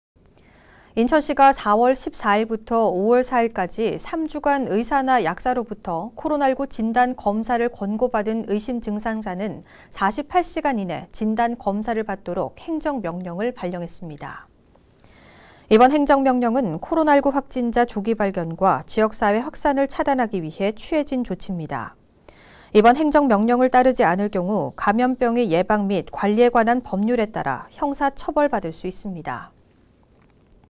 [NIB 뉴스] 인천시, 코로나19 검사 권고 의심증상자 진단검사 행정명령
인천시가 4월14일부터 5월4일까지 3주간 의사나 약사로부터 코로나19 진단검사를 권고 받은 의심증상자는 48시간 이내 진단검사를 받도록 행정명령을 발령했습니다.

이번 행정명령은 코로나19 확진자 조기 발견과 지역사회 확산을 차단하기 위해 취해진 조칩니다

이번 행정명령을 따르지 않은 경우 ‘감염병의 예방 및 관리에 관한 법률’에 따라 형사처벌 받을 수 있습니다.